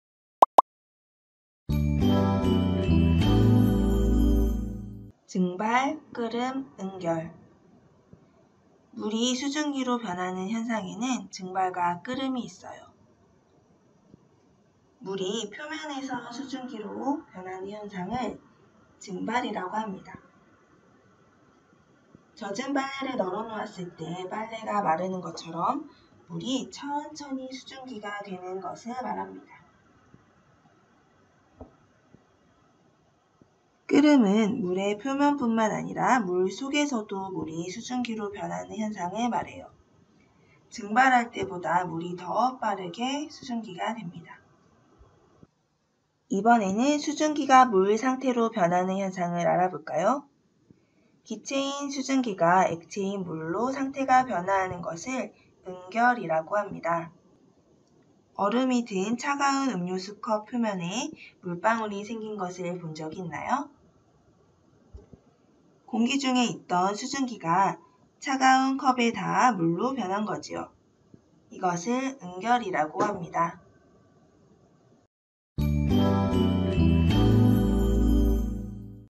[초능력 비주얼씽킹 과학] 4-에너지-17. 증발, 끓음, 응결
물질은 어떤 방법으로 상태가 변하게 될까요?
증발, 끓음, 응결을 알아봅시다.

▶ 동아출판 초등력 비주얼씽킹 과학 4학년